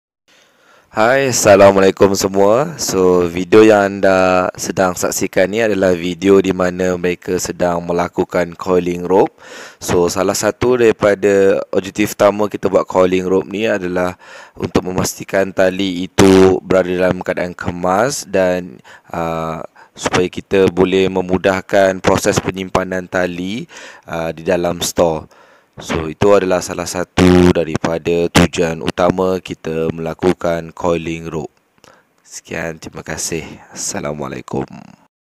Outdoor Education - Cara Untuk Menyimpan Tali (Coiling Rope)
Diantara fungsi 'Coiling Rope' adalah untuk proses penyimpanan tali di dalam stor dan ia sangat efektif bagi proses pengambilan tali semula jika kita hendak menggunakannya kembali.